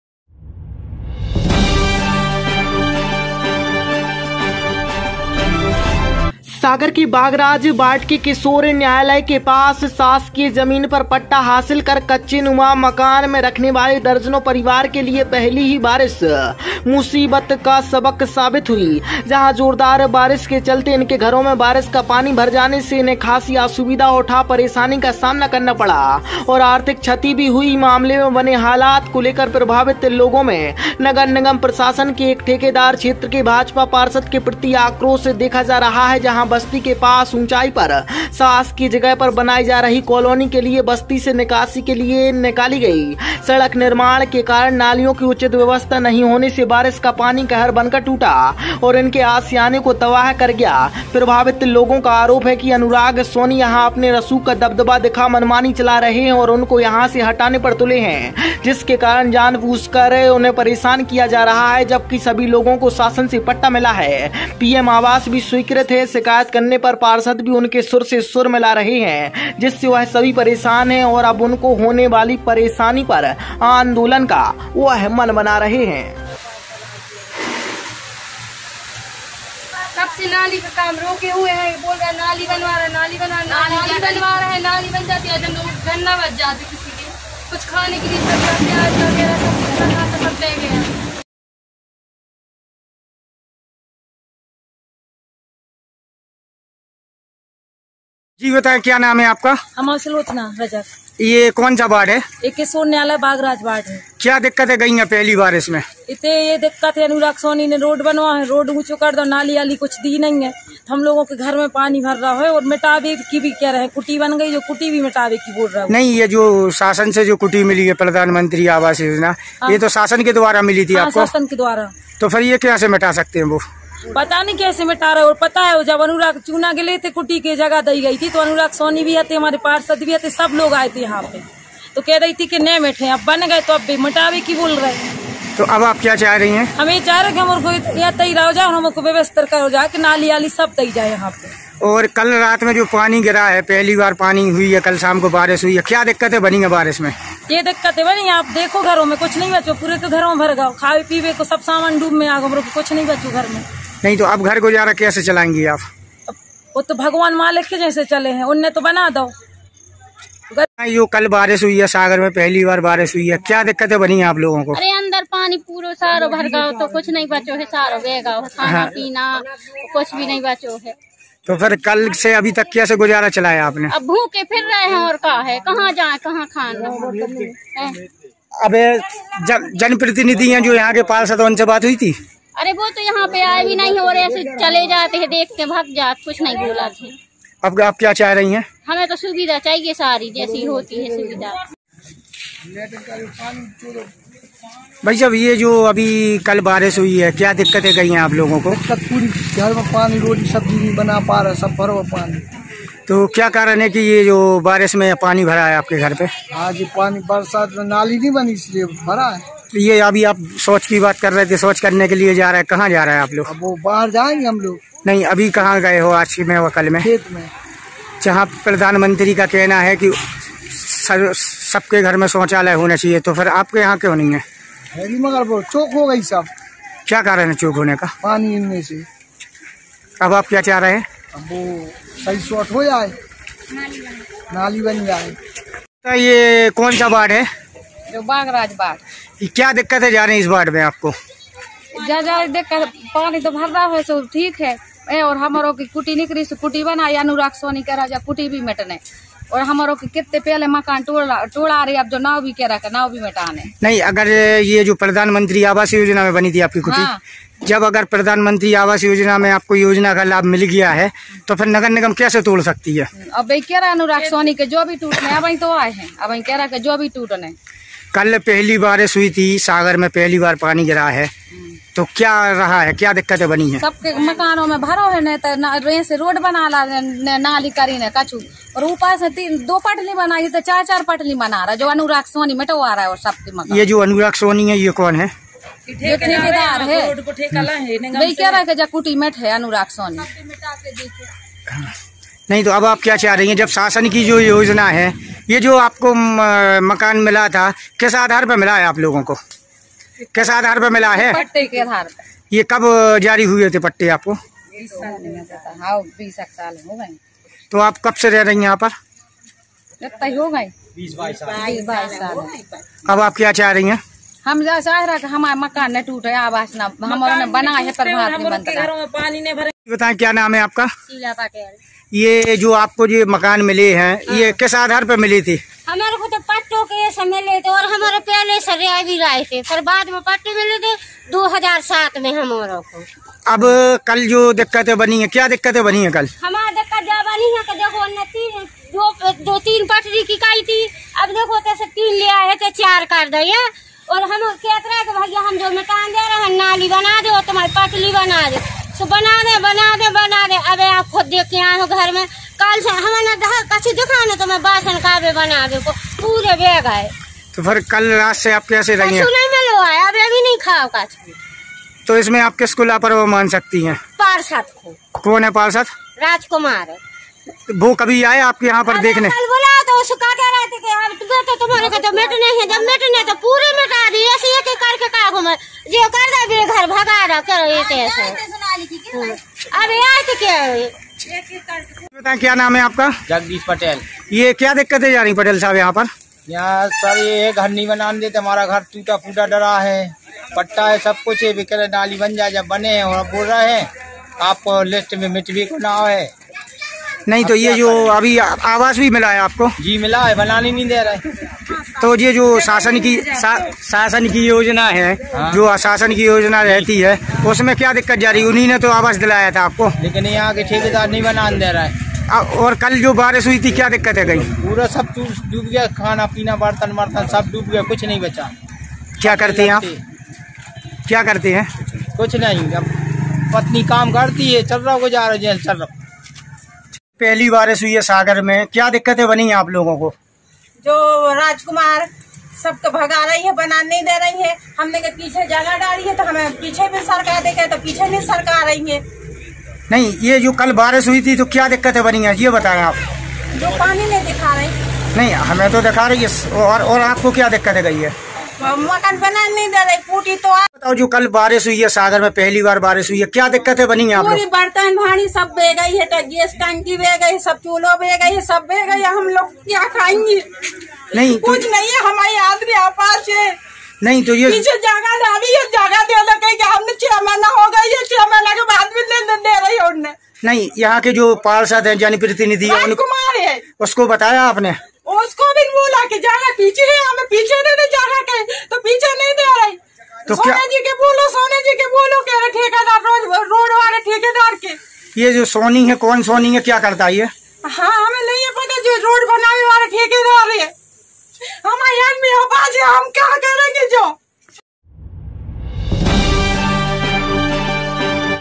बारिश का पानी कहर बनकर टूटा ओर इनके आशियाने को तबाह कर गया प्रभावित लोगो का आरोप है अनुराग सोनी
सागर के बाधराज वार्ड के किशोर न्यायालय के पास शासकीय जमीन पर पट्टा हासिल कर कच्चे नुमा मकान मे रखने वाले दर्जनों परिवार के लिए पहली ही बारिश मुसीबत का सबक साबित हुई जहां जोरदार बारिश के चलते इनके घरों मे बारिश का पानी भर जाने से इन्हें खासी असुविधा उठा परेशानी का सामना करना पड़ा ओर आर्थिक क्षति भी हुई